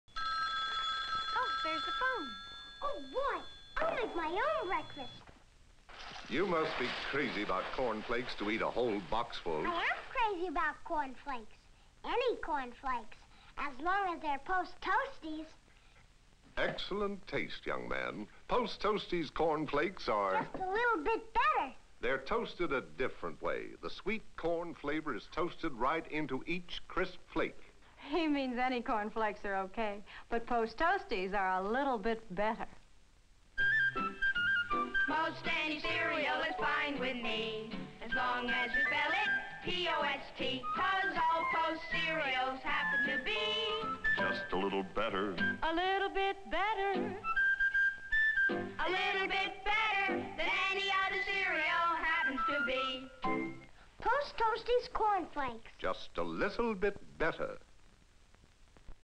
VINTAGE 1950s POST TOASTIES - LITTLE BOY WITH AN APPETITE BIGGER THAN JETHROE BODINE
Kellogg's had the "Beverly Hillbillies" character (Jethroe Bodine) as TV's resident HUMONGOUS PORTION breakfast cereal eater. Jethroe would use an incredibly huge baking bowl as his cereal bowl, and fill it with Kellogg's Corn Flakes. Prior to Jethroe being on TV, Post had an adorably cute little boy consuming a MASSIVE quantity of Post Toasties.

I hope this kid didn't continue eating such huge portions of Post Toasties. If he did, he would be carrying over 800 pounds of Post Toasties FAT on his body.

With a bowl of cereal that big, can you imagine the dump that was made?